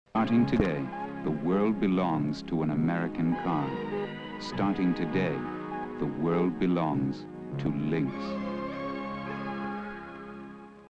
1981 Mercury Lynx Commercial- Short Ten Seconds
Very Short 10 second commercial for the Mercury Lynx